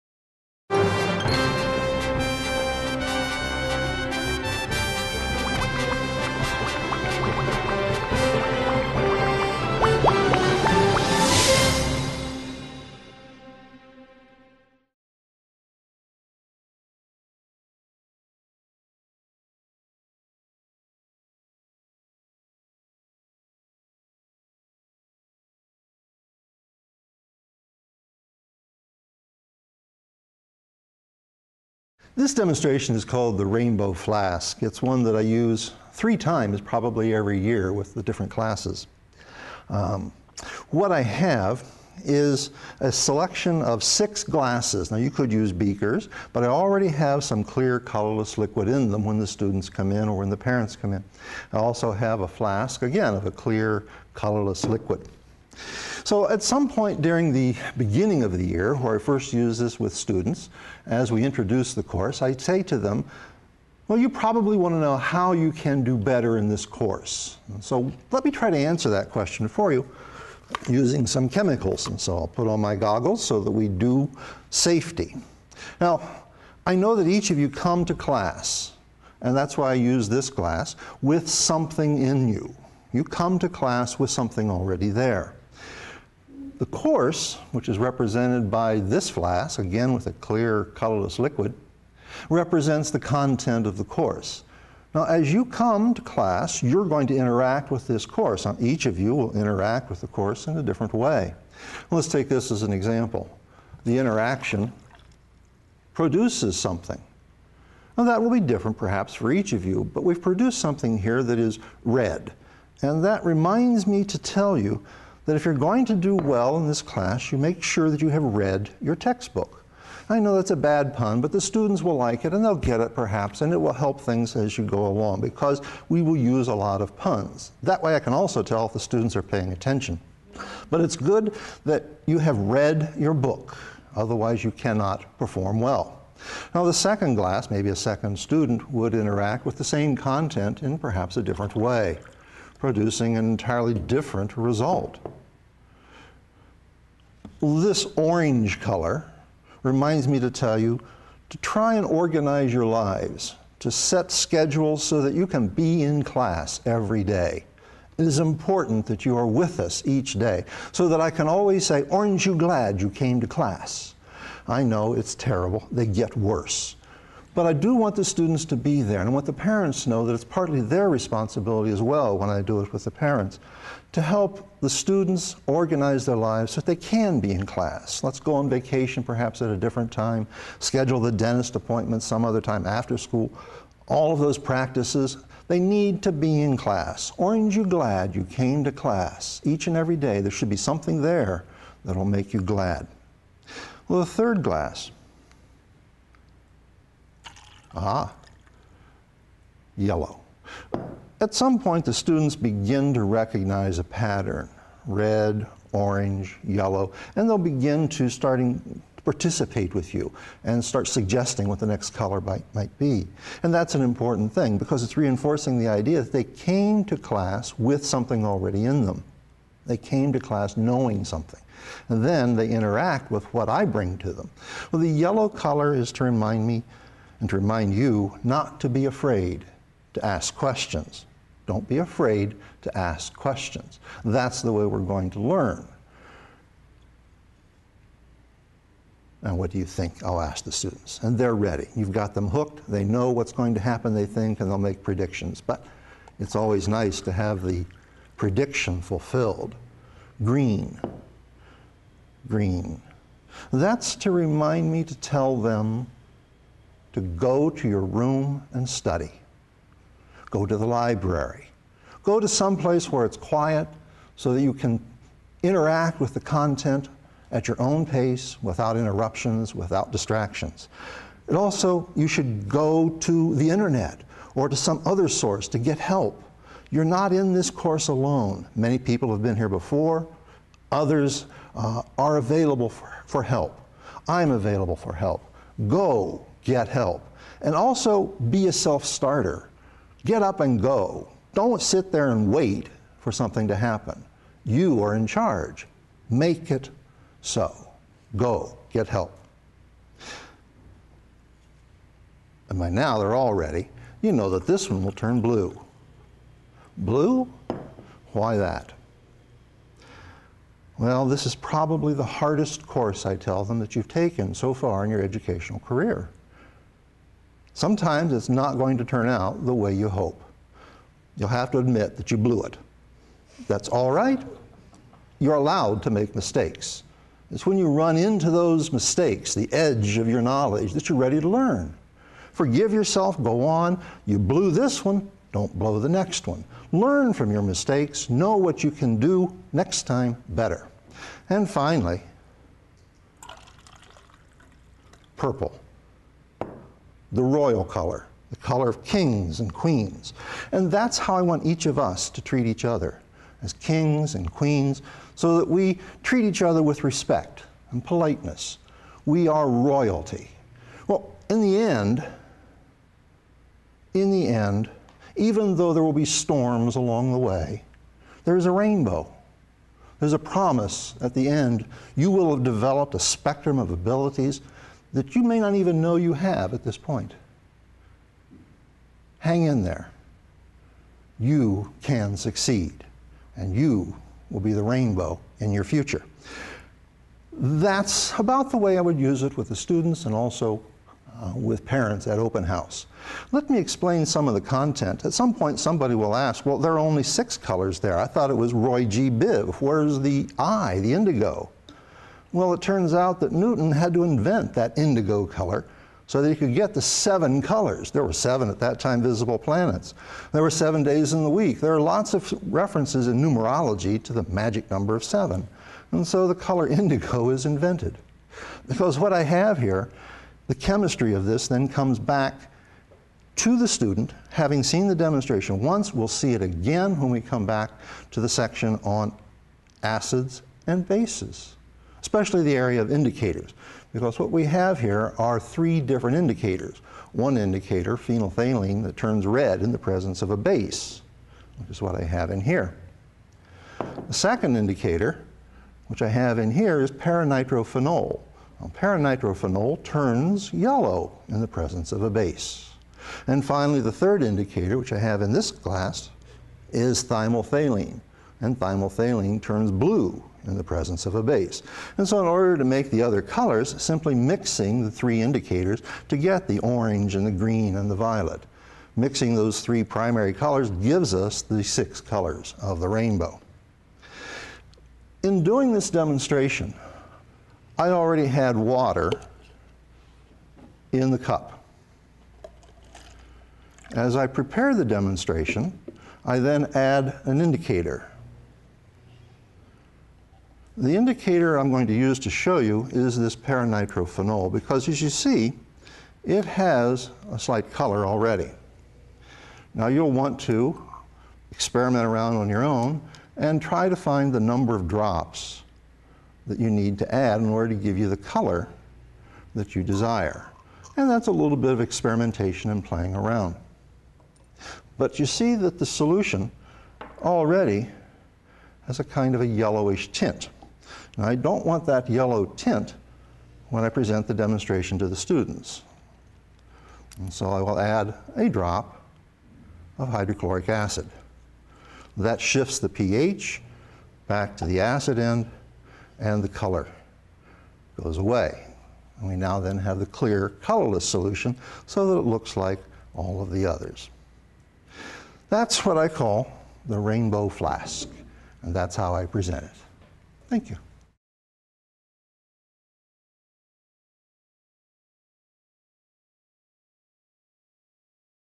Rainbow in a Flask | Teaching Chemistry
Create a beautiful rainbow of colors and share a powerful lesson to help students succeed in chemistry. Presented by Mike Roadruck as a part of the Flinn Scientific Teaching Chemistry Video Series, professional development for chemistry teachers. Over 125 additional video products are available